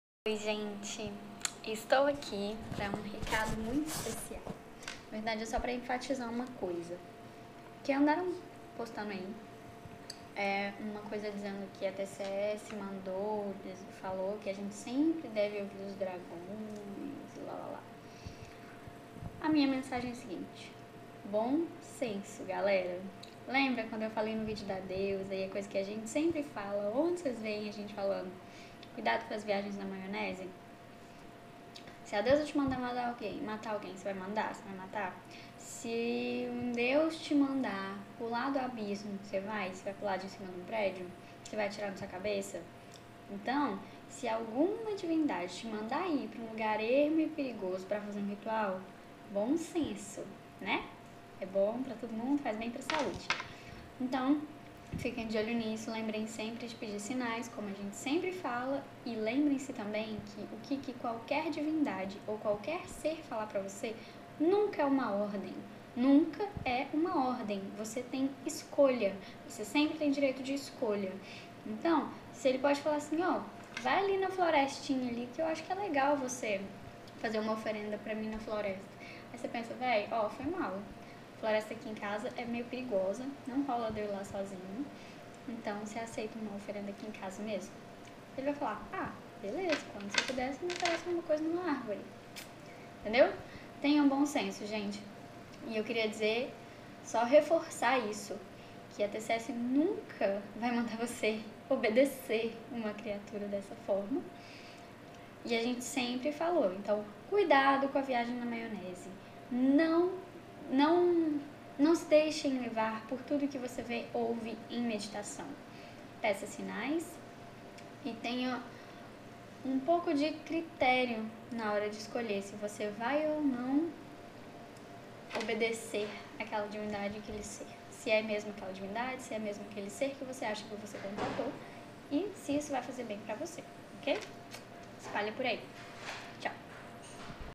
Bom senso, galera!
Uma das coisas mais importantes pra se ter em contato com divindades e seres mágicos (e vida)!

Deixamos claro que esta é a visão de nossa tradição, se vc não concorda, faça o que funciona para vc ou aquilo vc aprendeu em seu grupo ou tradição. Afinal, nossa religião não é dogmática, não temos nenhuma verdade escrita em pedra.

Também esclarecemos que ninguém da TCS é contra o cristianismo em qualquer vertente. Nós respeitamos todas as religiões e gostaríamos do mesmo respeito.